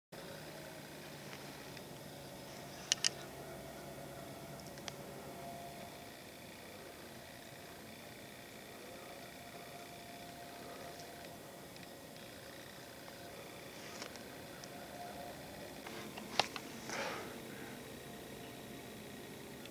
November - The Rut
Little clip i got of a nice buck coming into range, he surprised me so i couldn't really move. Just trying out some editing tricks.